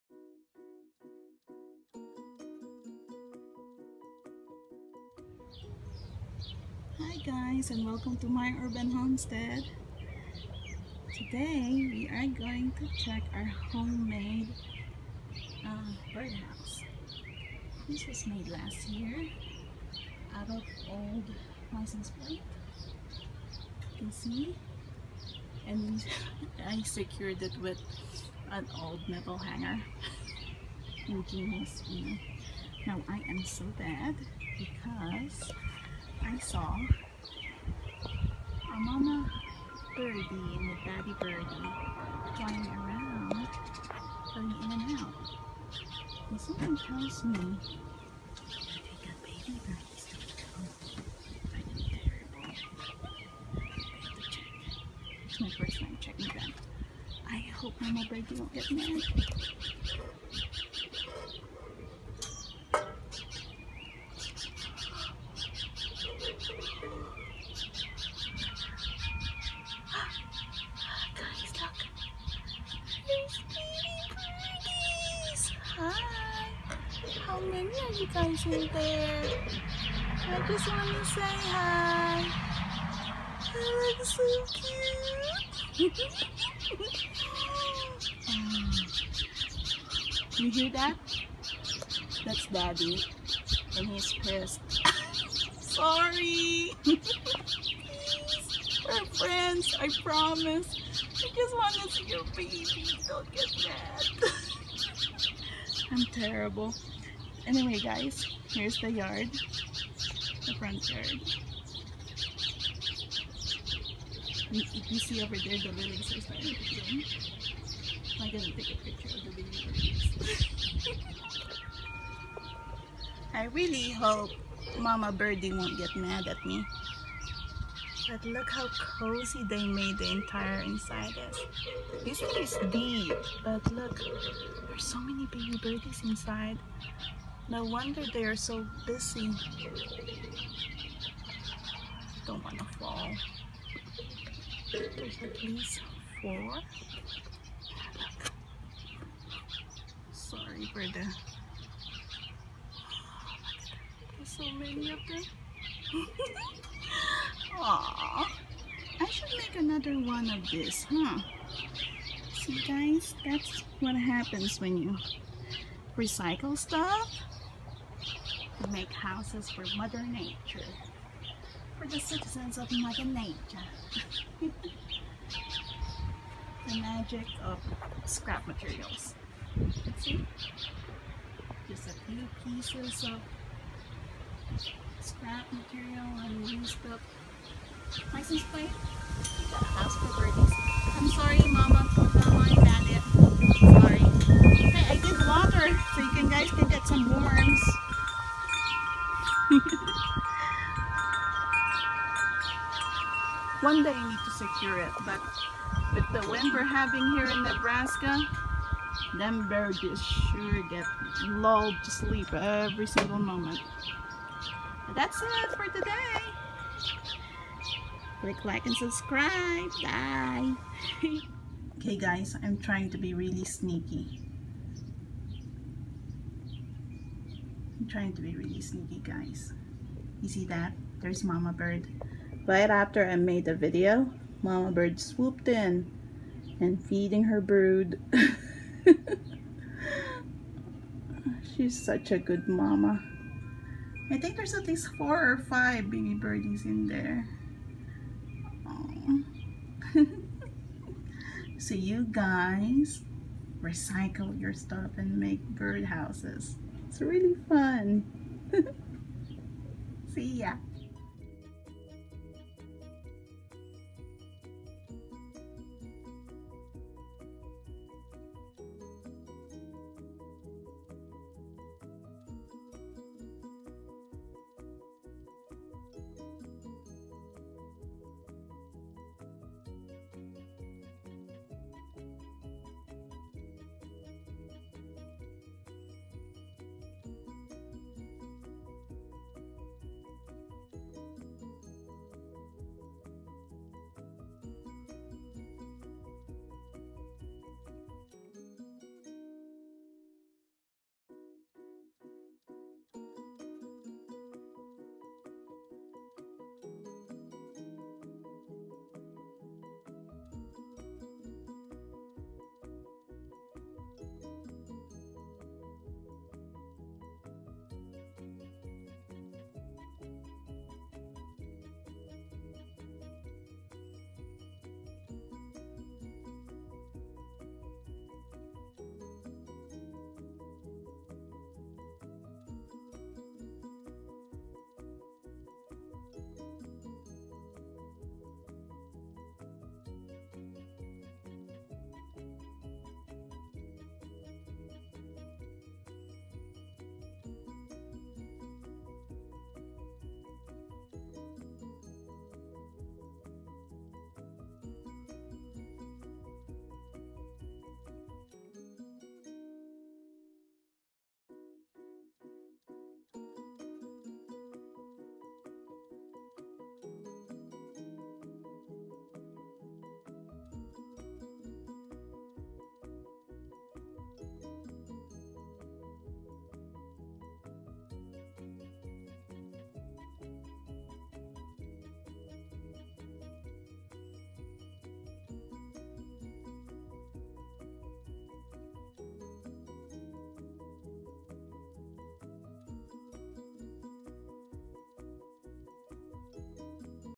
Birdhouse from Recycled Materials- became nest for House Sparrows /My Urban Homestead Midwest
Winter of last year, I made a birdhouse out of old license plates, scrap wood and metal hanger. Come early summer, I saw a pair of house sparrows checking it out. First week of summer they moved in and a family with 4 baby birdies 🐦 was born. Hope this video inspires you to make your own birdhouse using recycled materials. Have fun watching!